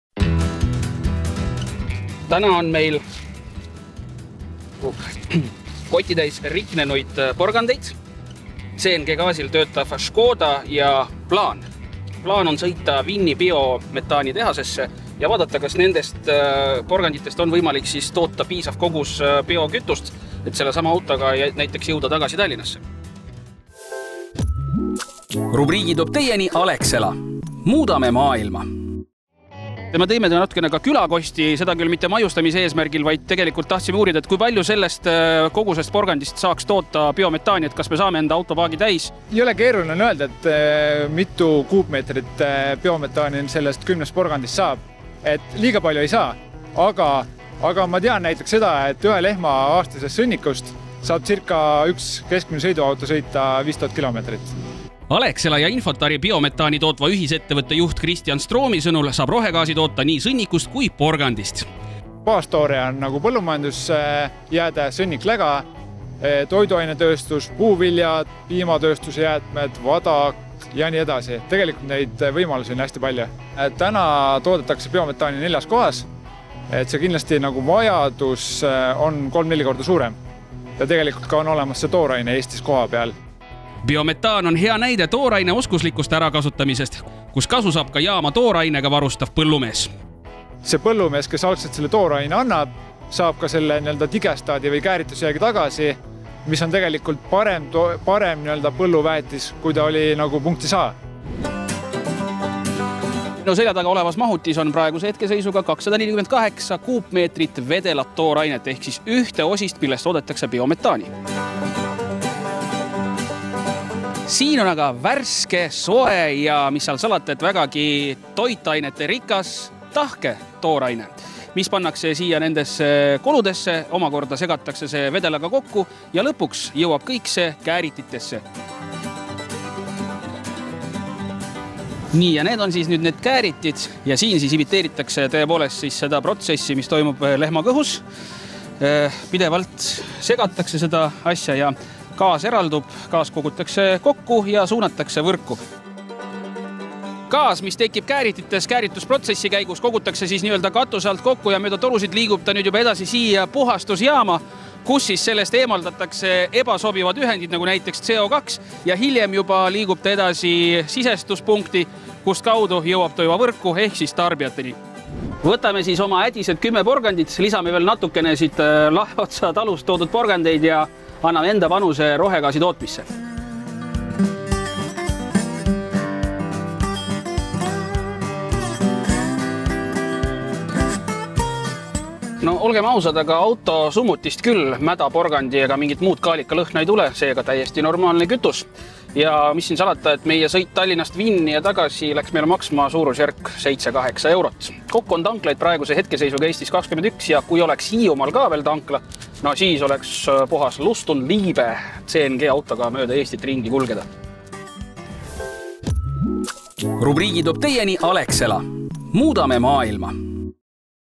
Lehmasõnnikust saab autokütust toota! Vaata juba videost 👀
Pakk porgandeid, Škoda CNG gaasiauto ja selge siht Vinni. Tule meiega kaasa ja vaatame, kuidas Alexela koos Infortariga Vinnis rohegaasi toodab. Muide, kas teadsid, et ühe lehma aastasest sõnnikust 🐄💩 saab üks keskmine sõiduauto sõita ca 5000 kilomeetrit 👀 Vägev!